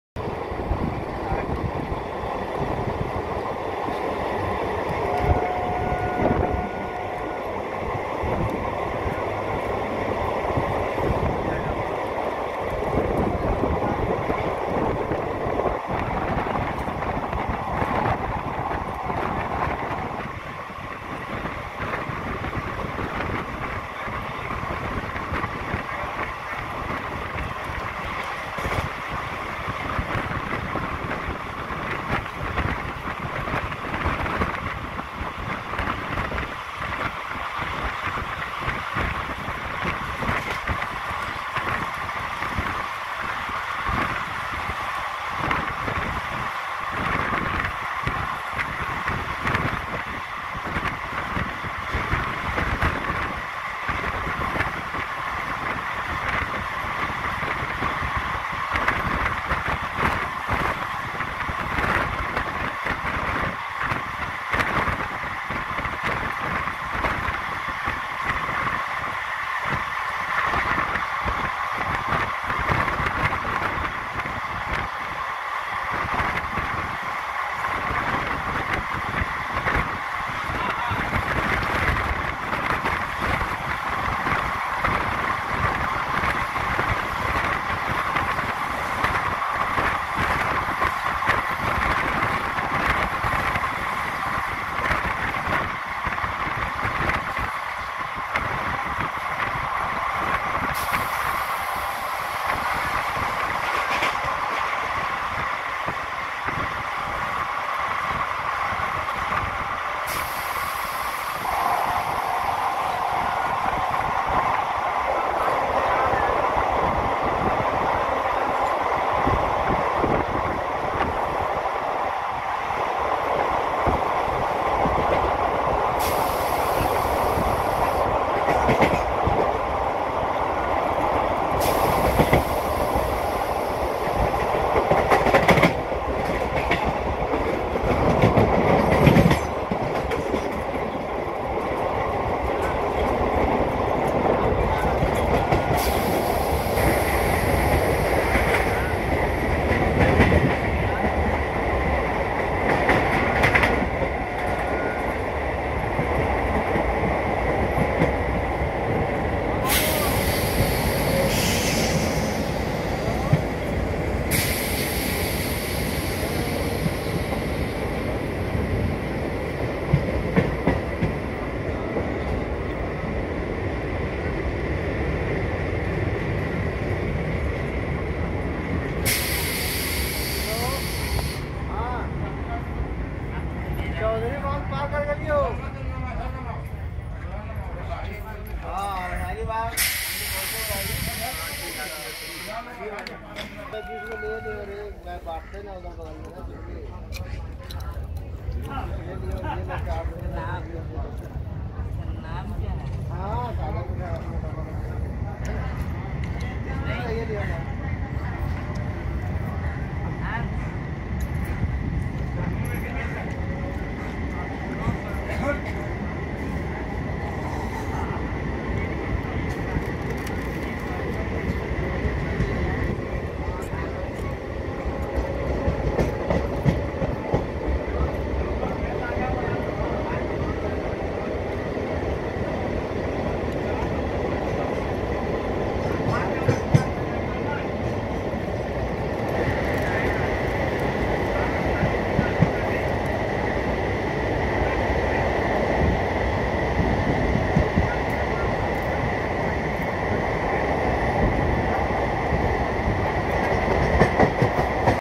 CCK, Chichaki Railway station Jharkhand, Indian Railways Video in 4k ultra HD
Chichaki Railway station is situated in Hankponk, Jharkhand. CCK, Chichaki Railway station, Indian Railways Video in 4k ultra HD